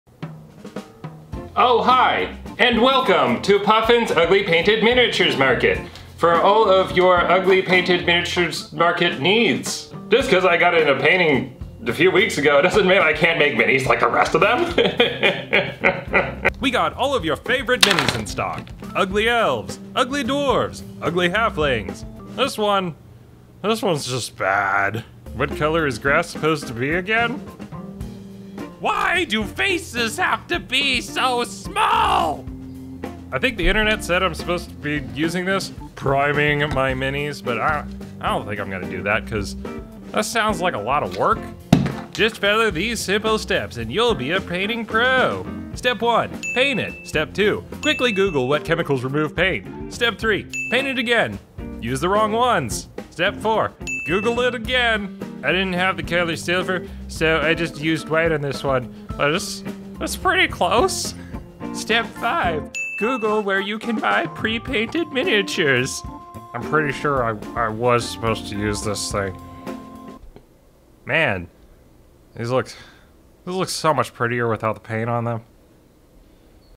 Puffin's Ugly D&D Miniatures || Getting into painting minis
Hey, just a short video. I got into painting minis and just wanted to talk a little bit about what it's like.

Note: I'm not ACTUALLY selling minis.